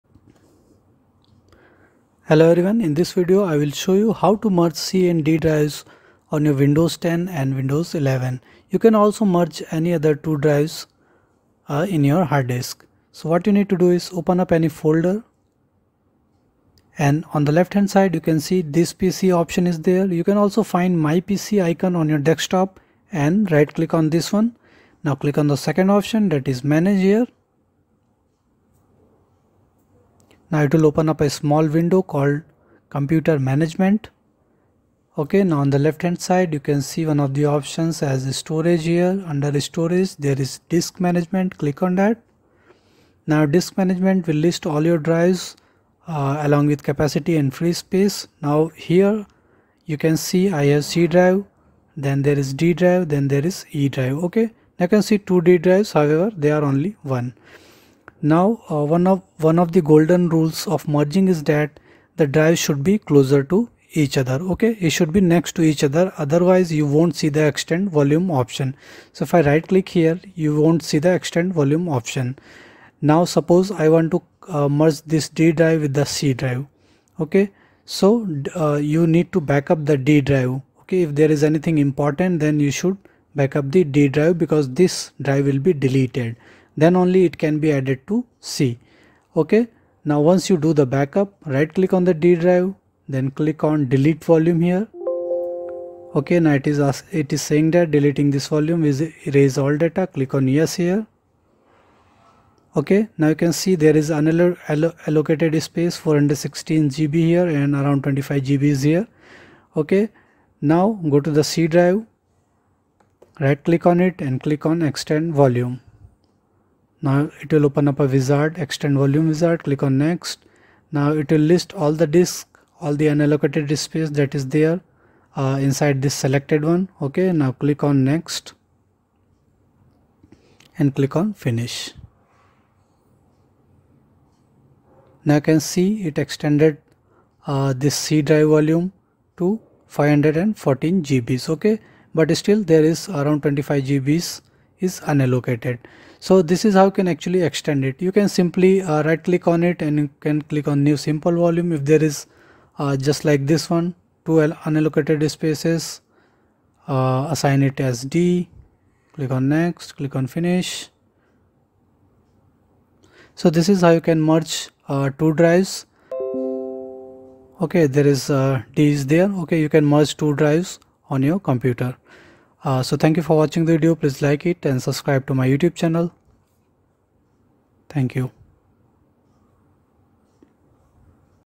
How to Merge C and D Drive in Windows 10/11
mergetwodrives combinedrives Windows10 Windows11 In this tutorial learn how to merge C and D drive in Windows 10 or Windows 11. You can also merge any other 2 drives on Windows. You should also know that 2 drives must be close to each other else extend volume option won't work.